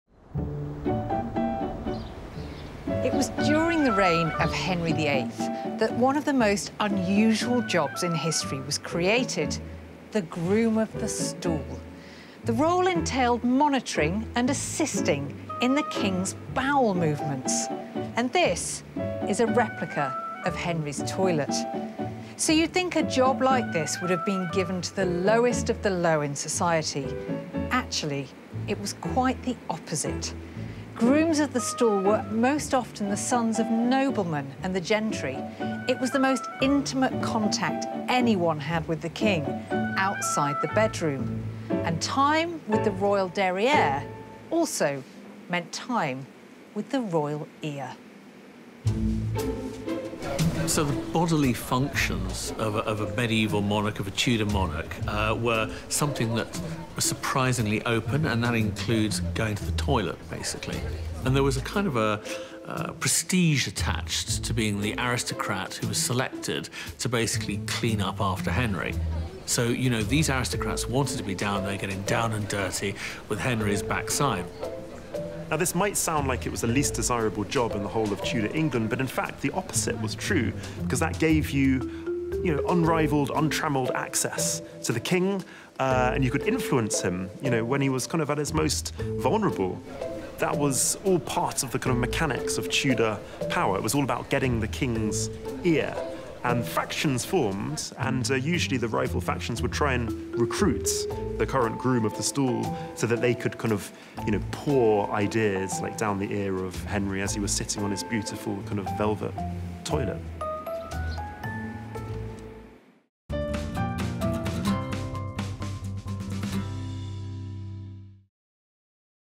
This Unpleasant Royal Job Came with a Fancy Title
It may sound like an auspicious title; but the ‘groom of the stool’ was anything but. Simply put, it was a role that entailed monitoring and assisting with Henry VIII’s bowel movements.